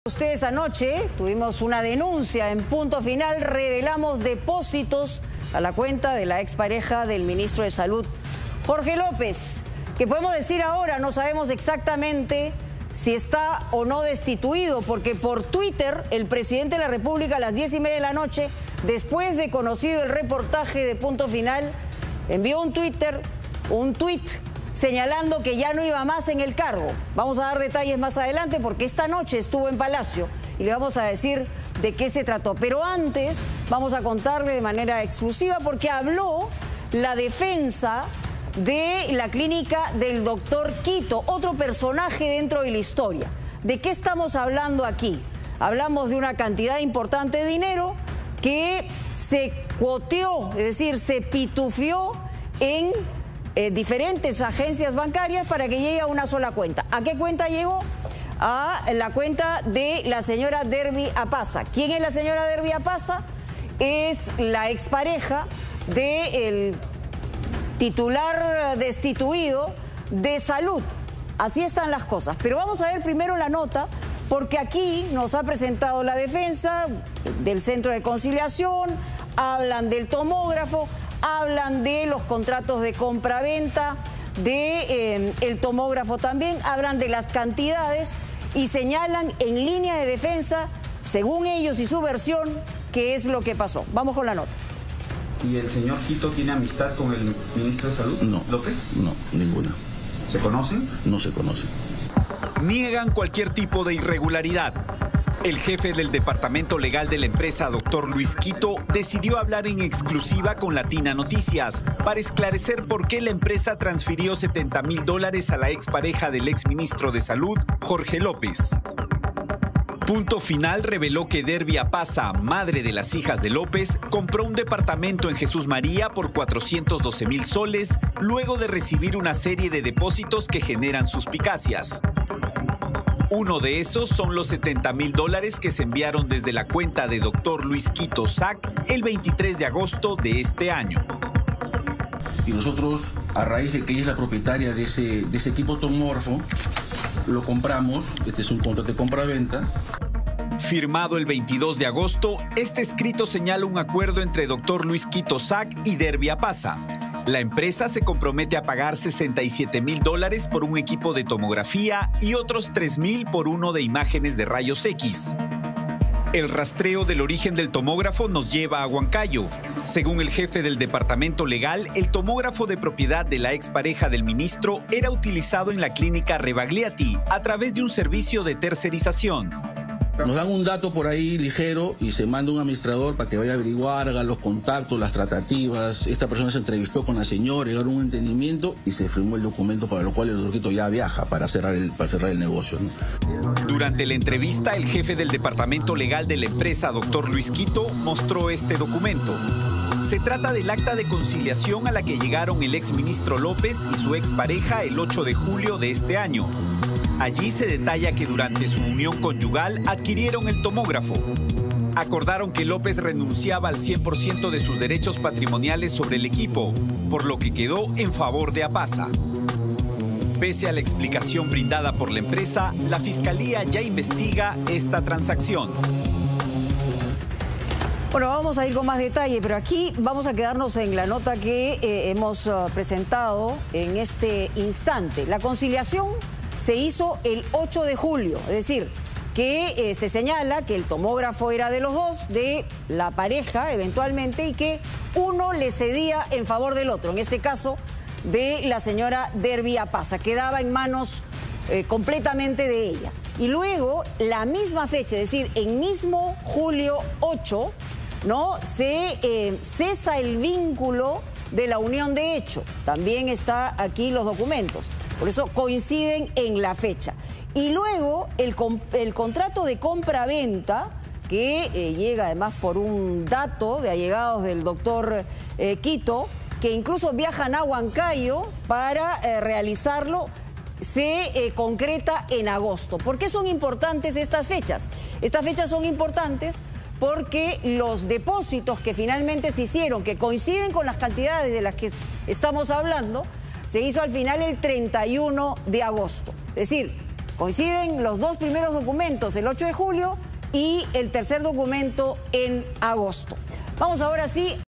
🔴 EXCLUSIVO - LA RUTA del tomógrafo de DERVY APAZA, la expareja del Ministro de salud, Jorge López
🔴 LNCentral EXCLUSIVO - Analizamos LA RUTA del tomógrafo de DERVY APAZA, la expareja del Ministro de salud, Jorge López.